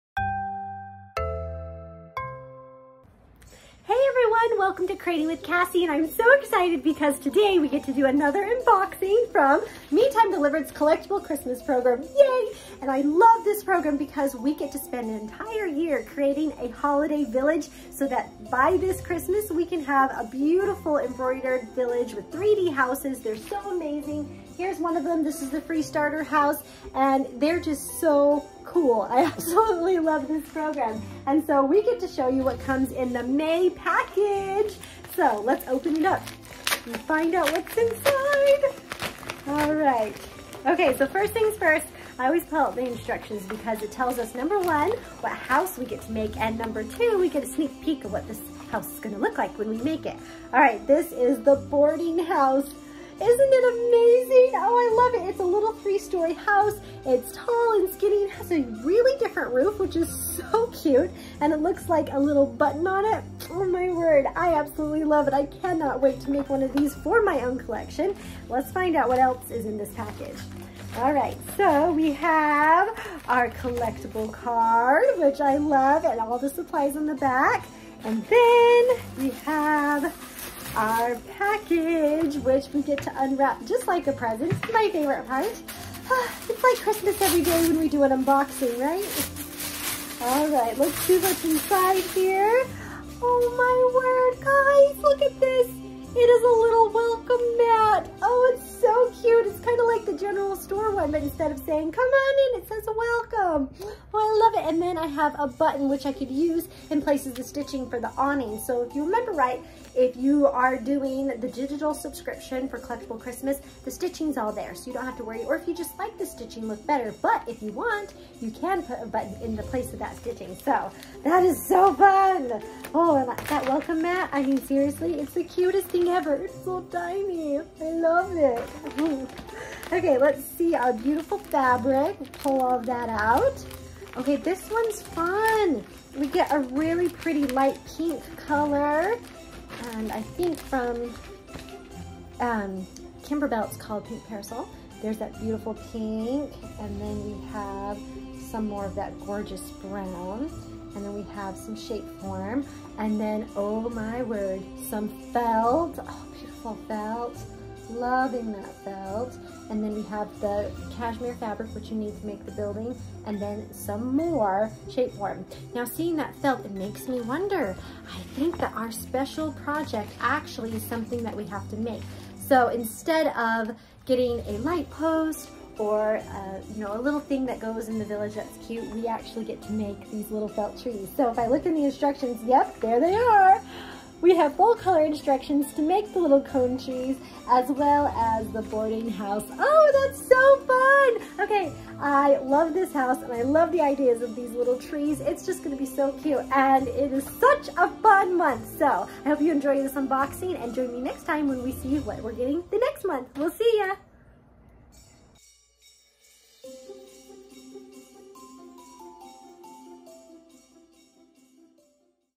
Unboxing the May 2023 Boardinghouse from ME Time Delivered | Collectible Christmas Holiday Village
IT'S TIME FOR AN UNBOXING!

In this video,  @creatingwithcassie shows you the Holiday Village Boardinghouse design from the ME Time Delivered Collectible Christmas subscription program and all of the super cool stuff that comes in the Package!

I hope you check out Collectible Christmas from ME Time and make some of your own.

You can check out ME Time Delivered and their subscriptions